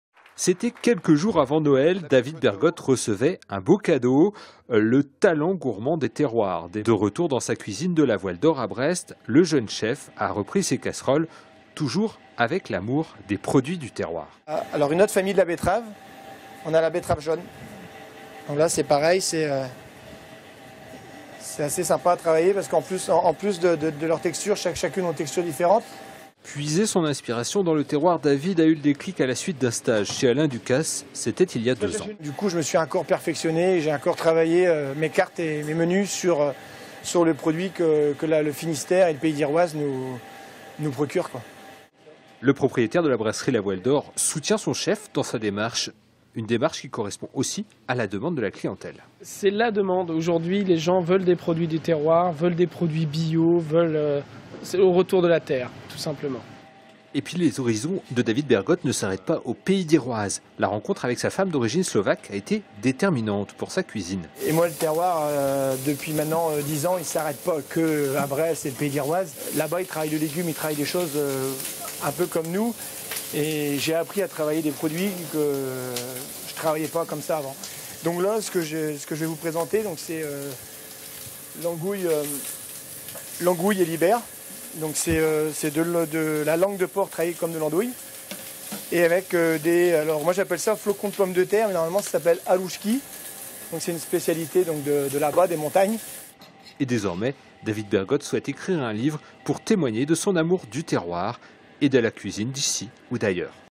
Portrait de David Bergot, un amoureux du terroir (Brest)
Sites webs/blogs, copiez cette vidéo avec le bouton "Intégrer" !
Portrait de David Bergot, lauréat du concours Talent Gourmand dans le terroir Finistère. Pour exploitation TV, droits réservés, nous contacter